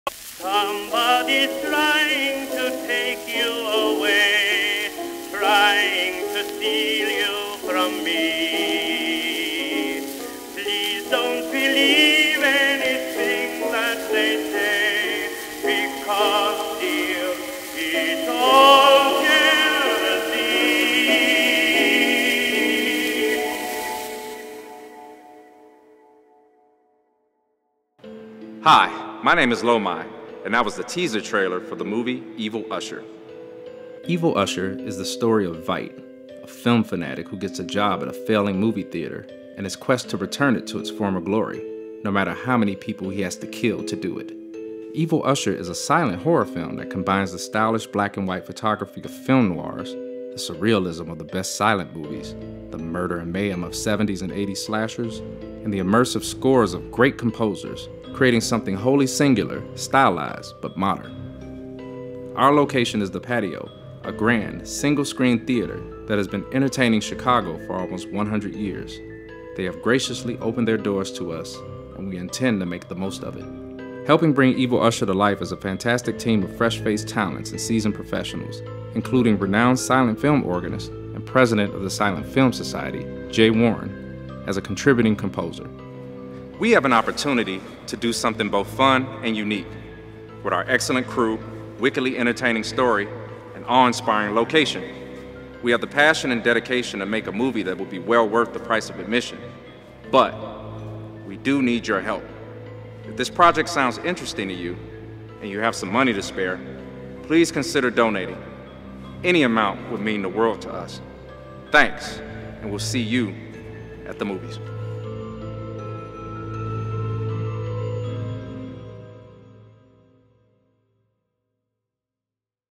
"Evil Usher" movie Short Pitch Video with Lomai
Writer/Director Lomai tells us why Evil Usher needs to be made Now. Check out our short pitch video for the feature film "Evil Usher".
A black and white, silent slasher film made for modern audiences.
We are currently in Preproduction and will shoot mid October 2024